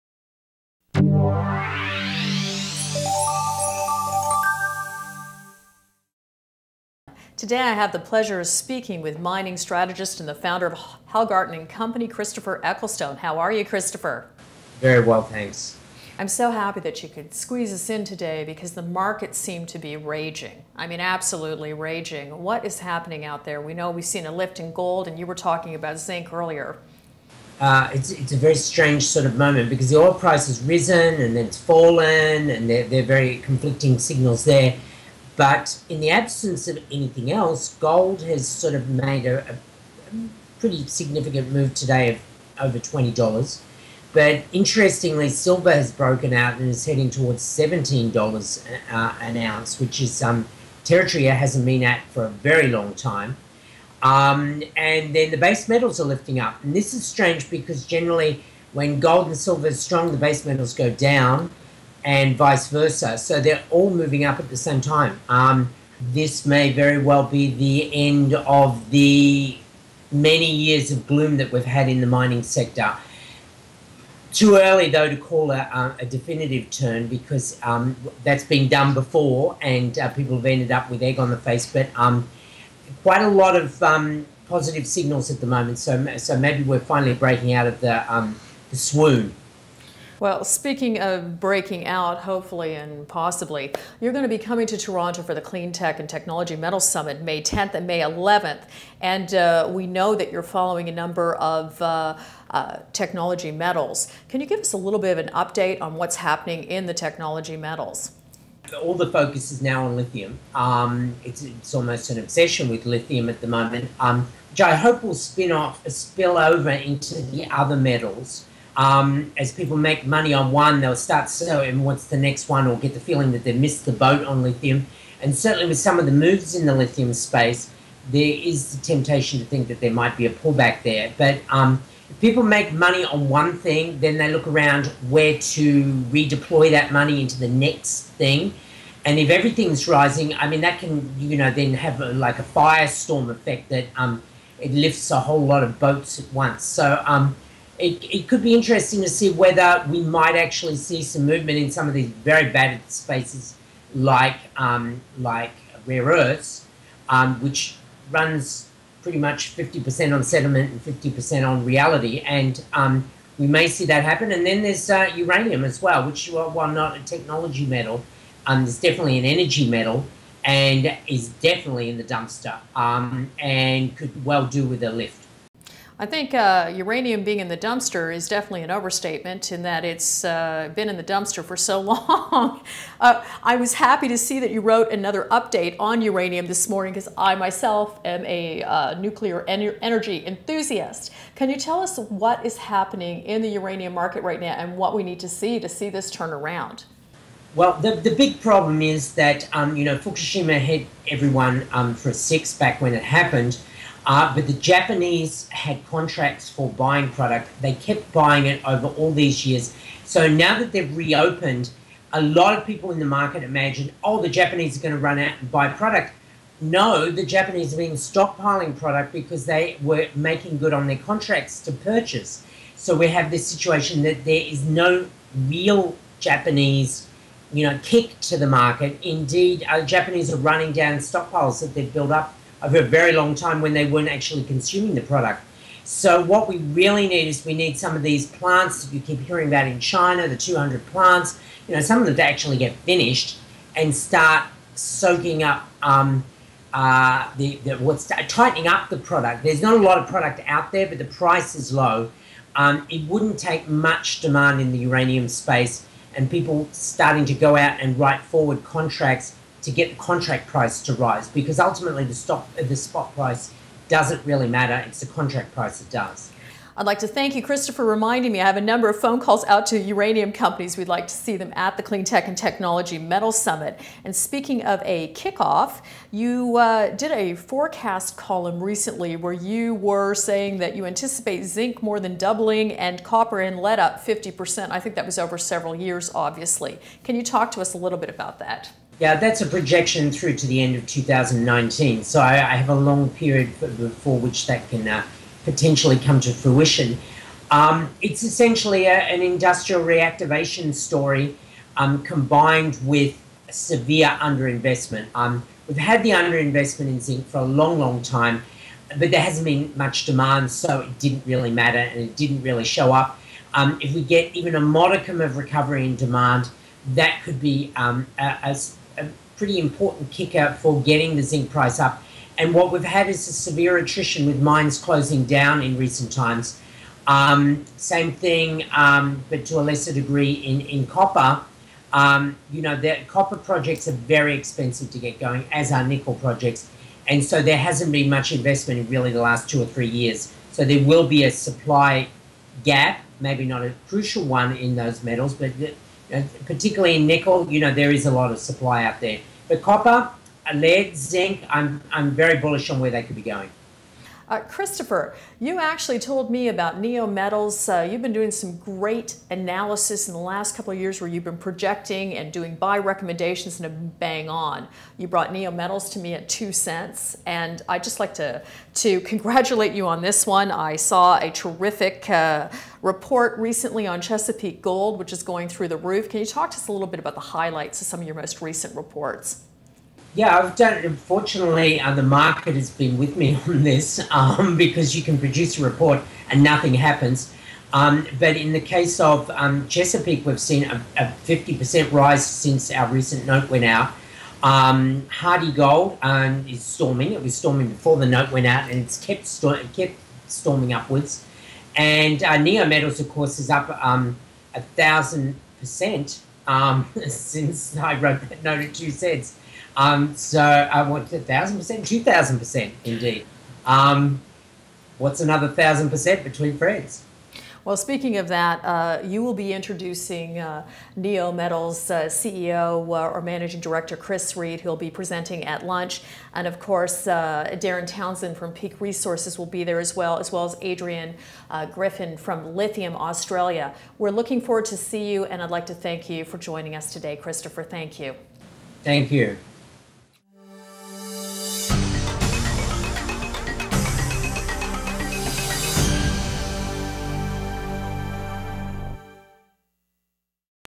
Ecclestone on the lithium phenomenon and the future for uranium
Christopher Ecclestone of London-based Hallgarten & Co is not afraid of bold calls. He was one of the first analysts in 2011 to suspect that the rare earths boom might not last too long. Lately he has made very bullish forecast for zinc, lead and copper.

He will be in Toronto for the Cleantech and Technology Metal Summit May 10th and May 11 -- to access the agenda, see below...

He hopes we may be seeing the end of many years of gloom that we’ve been having in the mining sector. He was encouraged last week by the fact that gold, silver and base metals all rose in unison. While Christopher is not suggesting the recovery is signed and sealed — too many people have made that mistake before — he is encouraged by the number of positive signals. “So maybe finally we’re breaking put of the swoon,” he adds.

In this interview with InvestorIntel publisher Tracy Weslosky, Christopher discusses his market outlook, especially the lithium phenomenon. He tells us how
◾Sees some future for uranium; stocks are tight and all it needs is for China to finish building the many reactors now in construction
◾Explains how industrial reactivation, along with severe underinvestment in recent years, is setting the scene for a zinc breakout

Tracy Weslosky: We know you’re following a number of technology metals. Can you give us a little bit of an update on what’s happening in the technology metals?

Christopher Ecclestone: All the focus is now on lithium. It’s almost an obsession with lithium at the moment, which I hope will spinoff — spill over into the other metals. As people make money on one, they’ll start saying, “what’s the next one?” or get the feeling that they’ve missed the boat on lithium. Certainly with some of the moves on the lithium space there is the temptation to think that there might be a pullback there, but people make money on one thing then they look around where to redeploy that money into the next thing. If everything is rising, I mean, that can have like a firestorm effect. It lifts a whole lot of boats at once. It could be interesting to see whether we might actually see some movement in some of these very battered spaces, like rare earths, which runs pretty much 50% on sentiment and 50% on reality. We may see that happen and then there’s uranium as well, which you could argue whether or not it’s a technology metal. It’s definitely an energy metal and is definitely in the dumpster and could well do with a lift. To access this video, click here ….